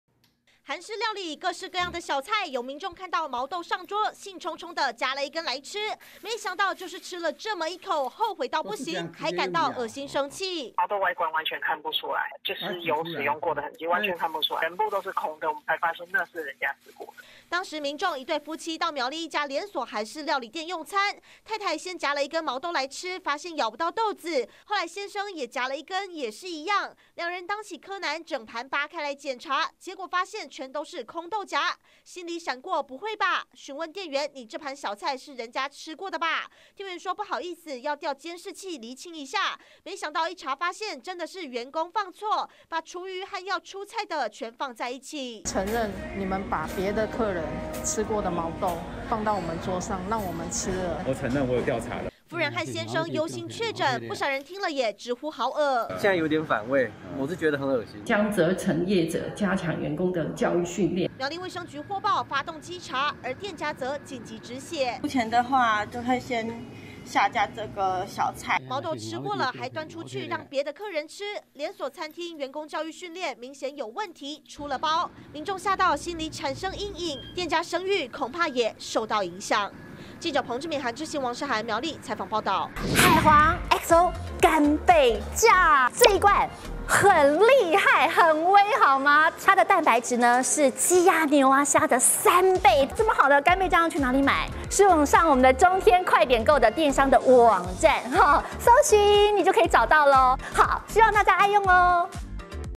【每日必看】噁! 毛豆吃到"空豆莢" 真相竟是"別人吃過的" 20221222
這真的太噁心了!苗栗一對夫妻，日前到韓式餐廳用餐，結果兩人都吃到了沒有豆子的空豆莢，仔細檢查發現，竟然整盤毛豆「全是空的」，趕緊詢問店家，結果令人崩潰，因為這居然是前面客人吃過的空豆莢。